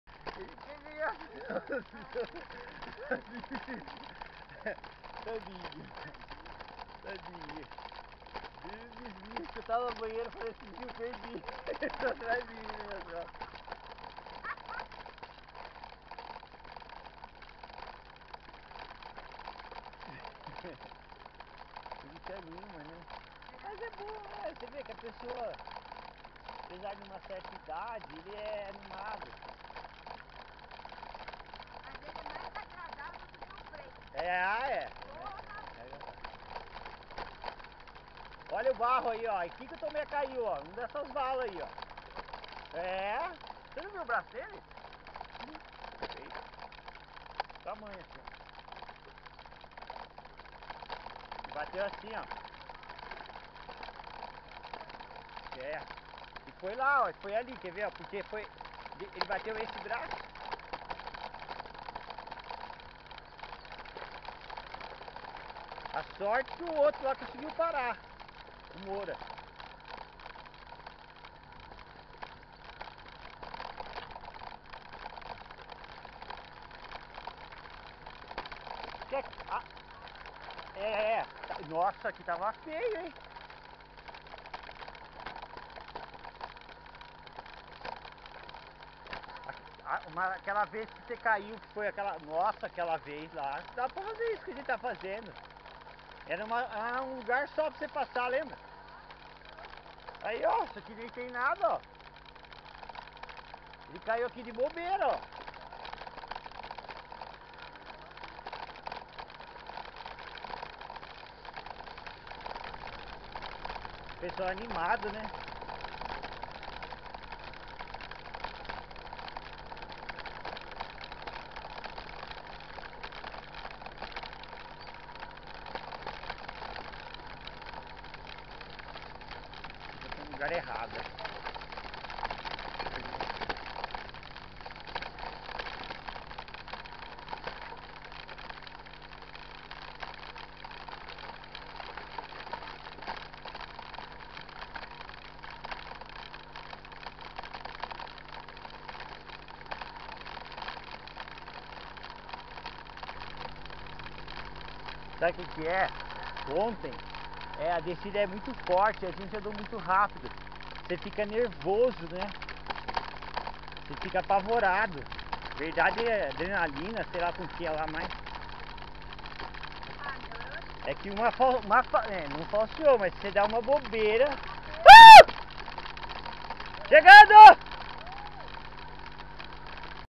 Mtb, Serra da Mantiqueira, Domingo Biker, 12 amigos, 58 km, 17
Taubaté, 05 de fevereiro de 2017.